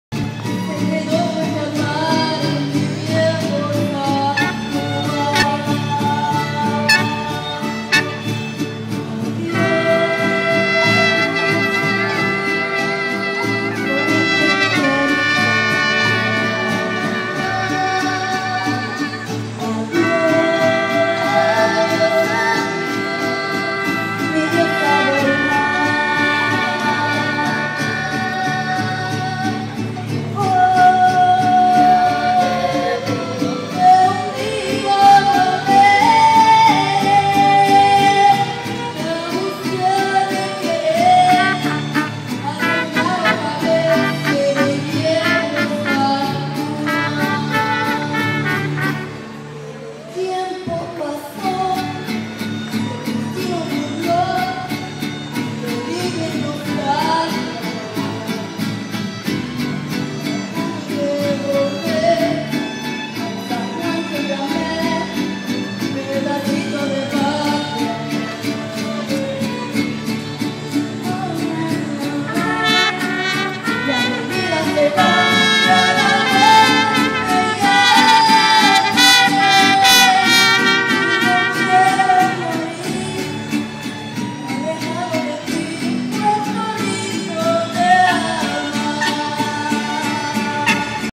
Fiesta Mexicana in Hanford
Paulina Medina performs for a packed house during the Fiesta Mexicana at the Hanford Civic Auditorium on Saturday evening. (iPhone video by Gary Feinstein/The Sentinel)